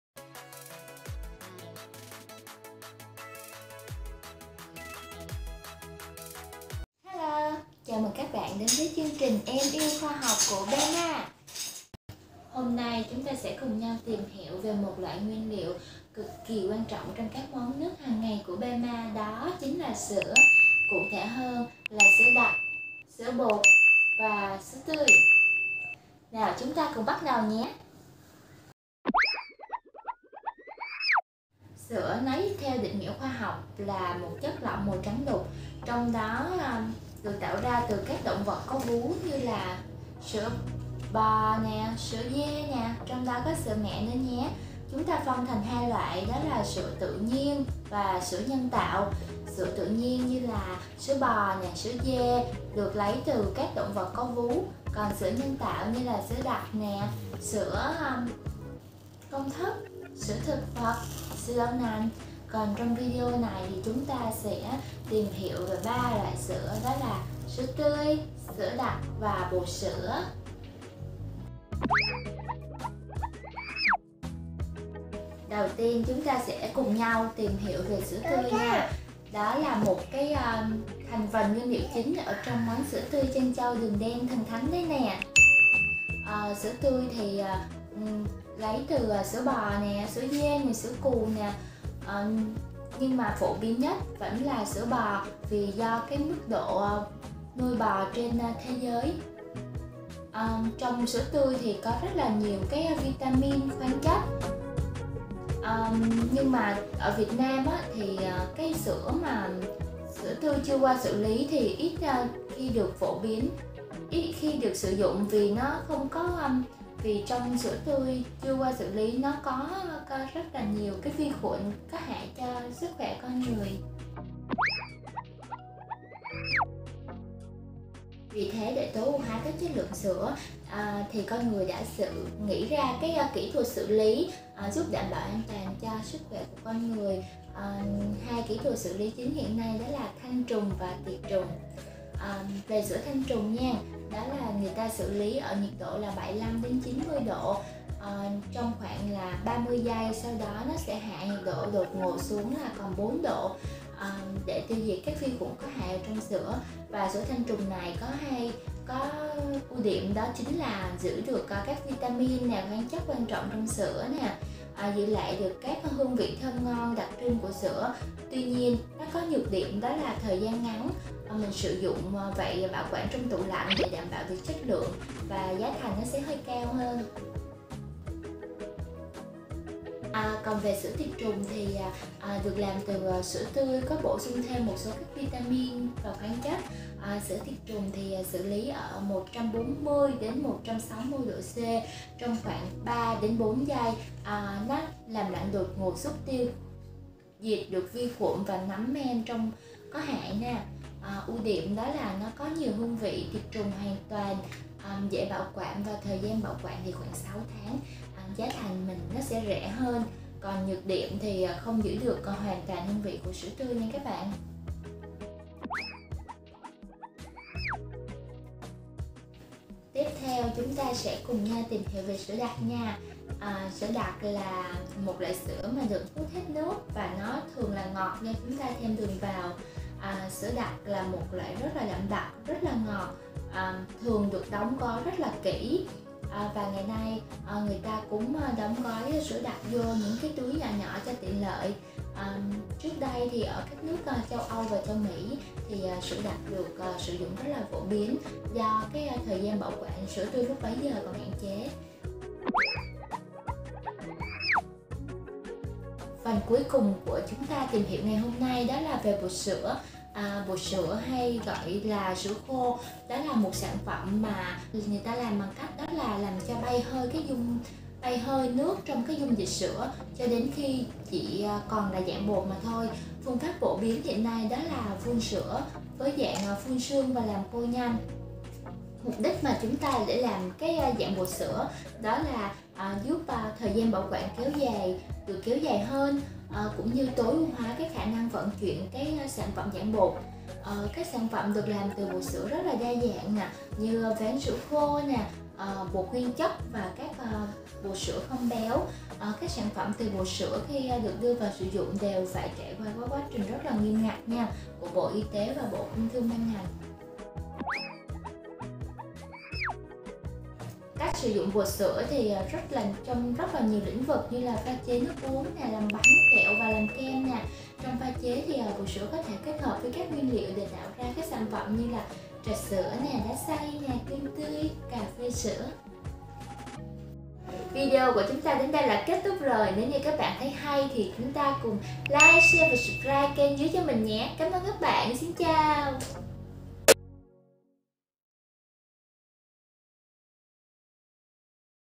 BEMA Vlog - Tìm hiểu về Sữa và các sản phẩm từ Sữa dùng trong pha chế
- Cùng BEMA tìm hiểu về Sữa, các sản phẩm từ sữa dùng trong pha chế nhé!
- Trà sữa BEMA. Địa chỉ 12C Lưu Hữu Phước, khu vực 4, phường 1, tp Vị Thanh, tỉnh Hậu Giang.